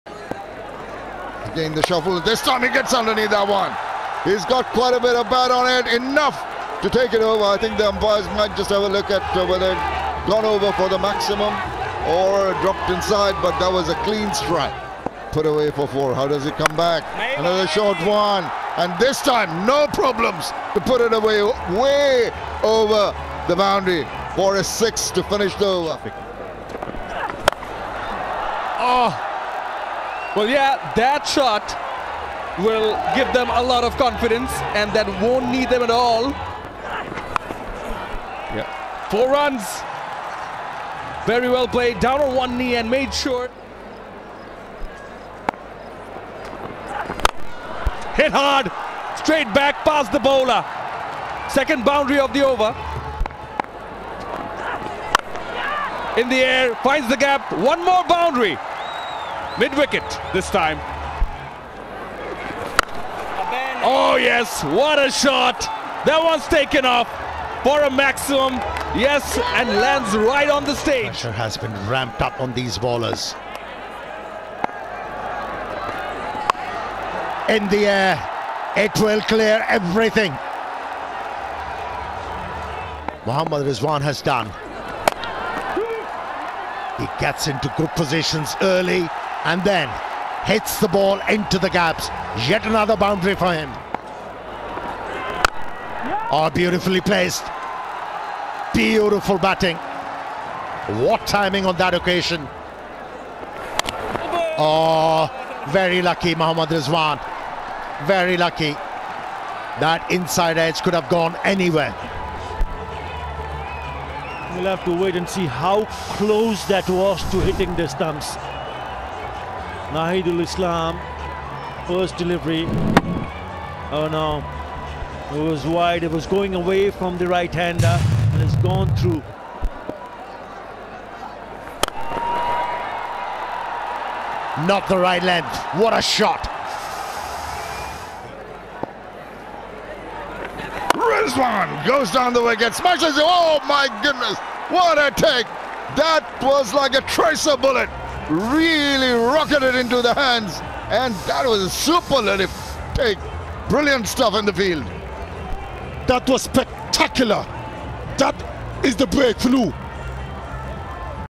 🔥 Muhammad Rizwan 73 Runs Off 39 Balls Against Khulna Tigers
Muhammad Rizwan 73 Runs Off 39 Balls Against Khulna Tigers BPL 2023 Match 32
Khulna Tigers vs Comilla Victorians